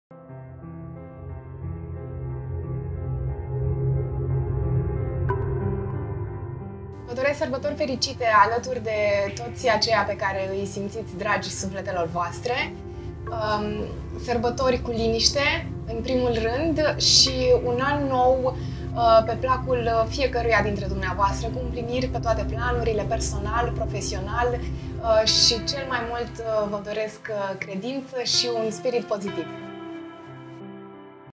Mara Mareș, președintele TNL, mesaj de sărbători pentru cititorii PSnews
Echipa PSNews, împreună cu colaboratorii noștri de peste an, vă urează tuturor Sărbători Fericite și un An Nou cu bucurii!